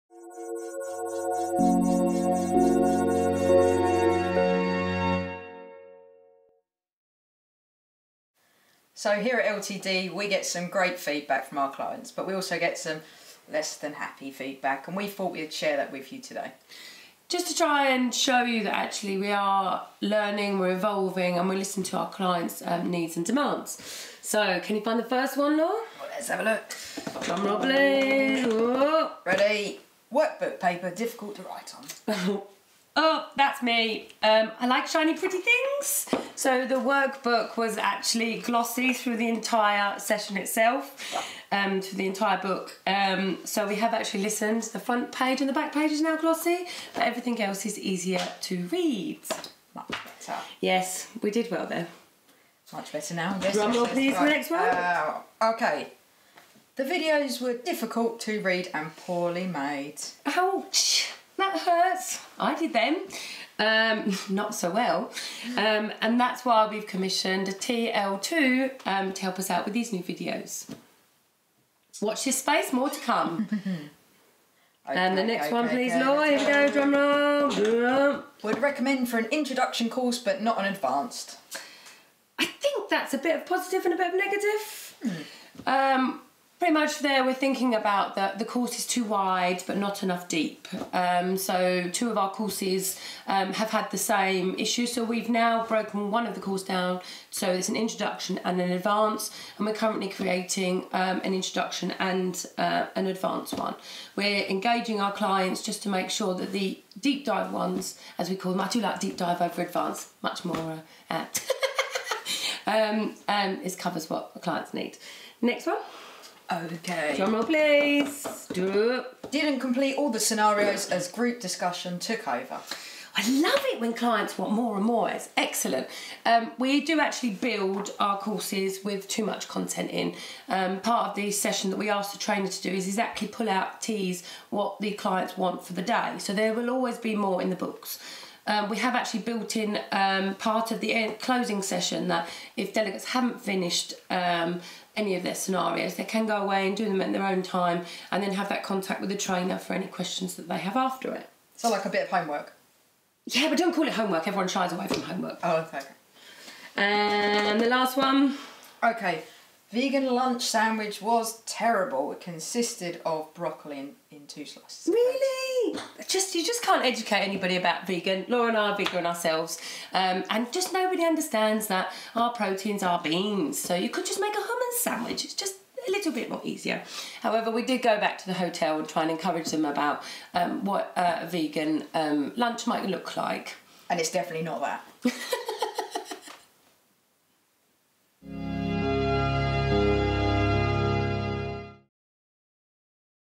Ep: 4 How we responded to Negative Feedback
A short video about how we have responded to negative feedback